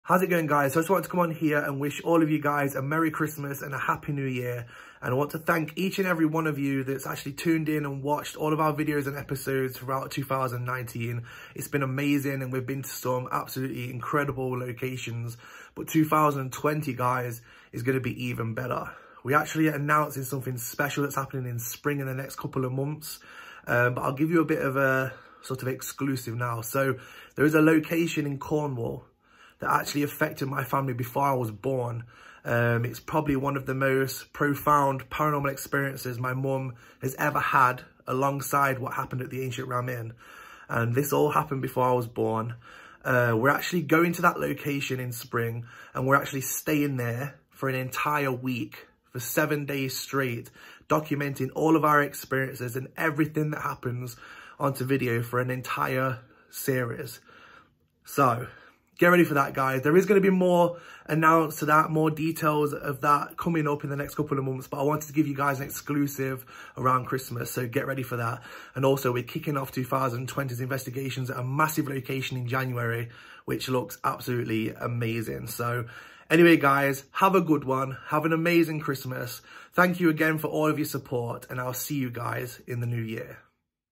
THIS WILL BE ABSOLUTELY TERRIFYING!
In 2020 we will be spending an entire week at a location in Cornwall where my family had one of there most profound paranormal experiences before I was born and every single thing is being documented onto video for an entire series.
Get ready for what will probably be one of our scariest paranormal series to date...

Have a great Christmas everyone and see you in the new year!